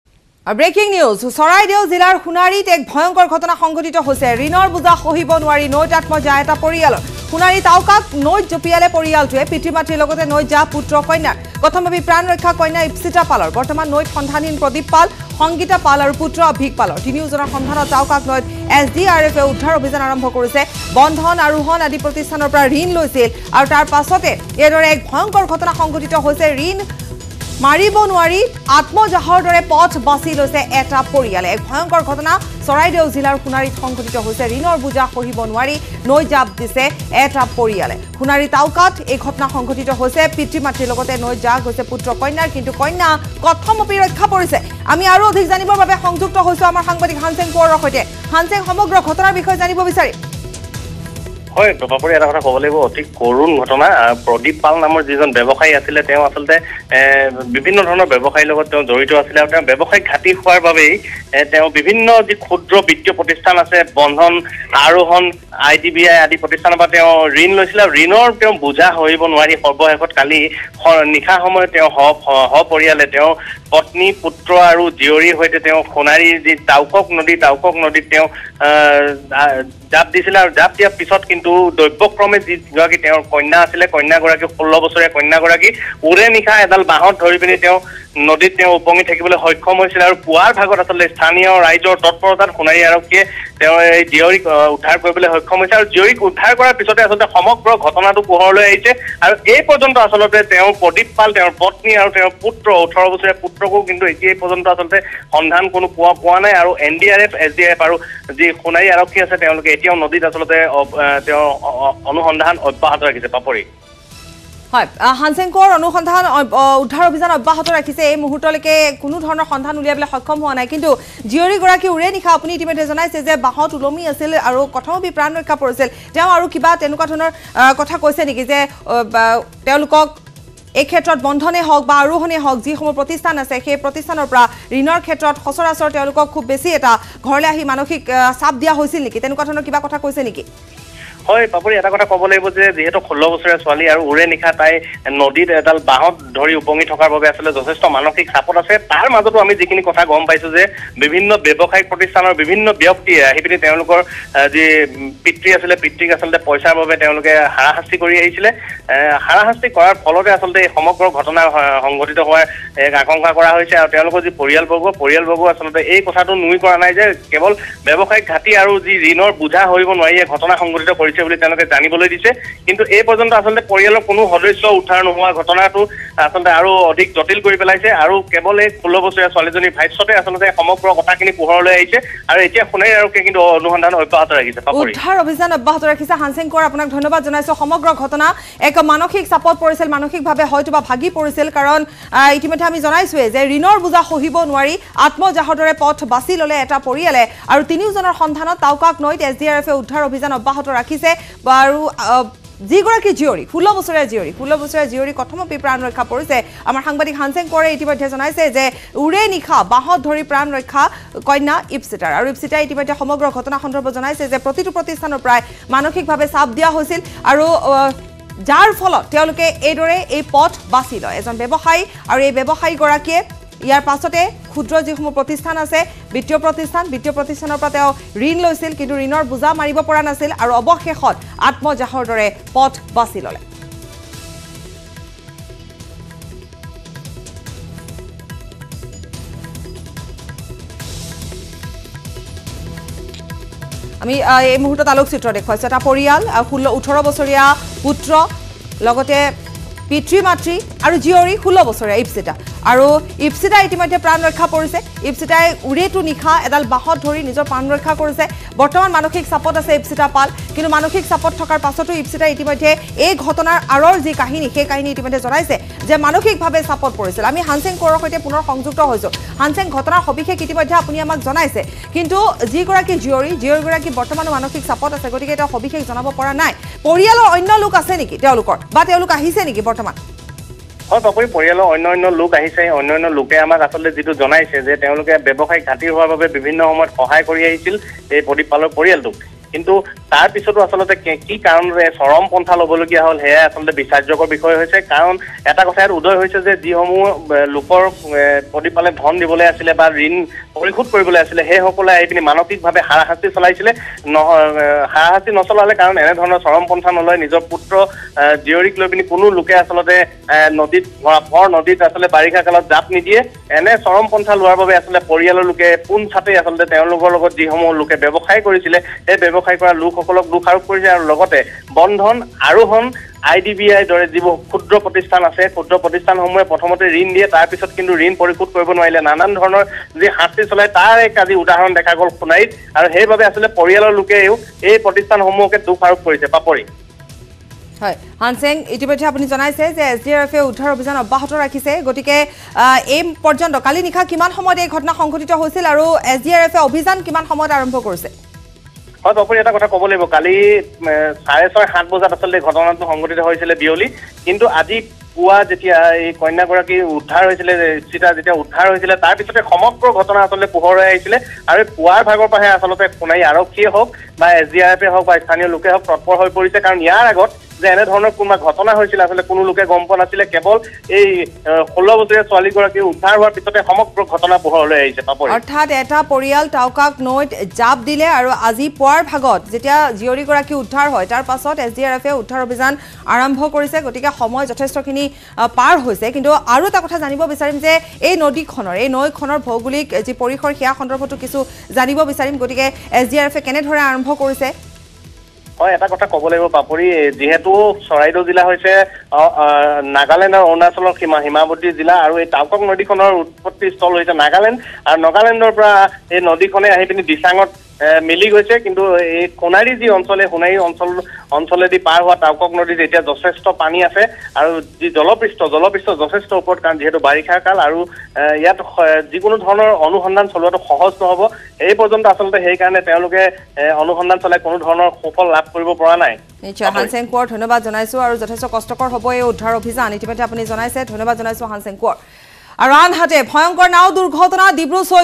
Family In Sunari Committed Suicide After Failing To Repay Debt
A family that had to take loans and credits in Sonari failed to repay the debts. The debts kept increasing and the pressure of repaying the debt kept increasing. As there was no way to repay the debt entire family, husband, wife, daughter, and son jumped into the river Brahmaputra. A search operation is going on to find the bodies.